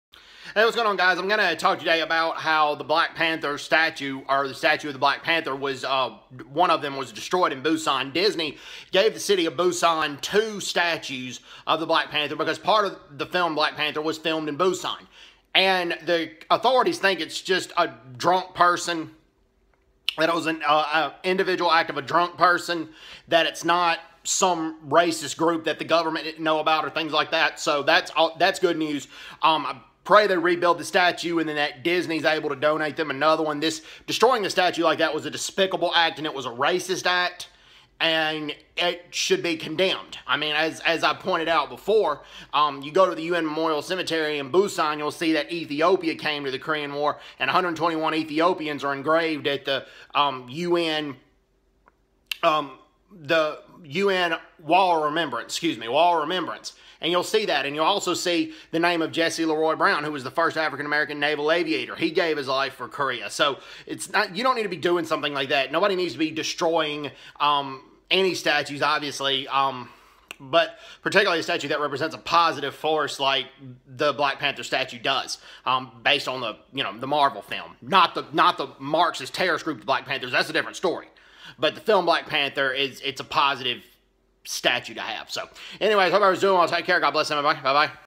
Black Panther statue destroyed in Busan
This looks like the act of one drunken racist individual but it still shouldn't have happened!!!